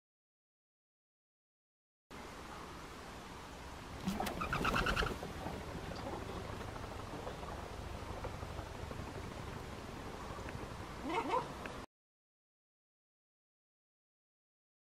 Ringed Teal Habitat
Centro de Reprodução de Anatideos. Ringed Teal Flying in an enclosure.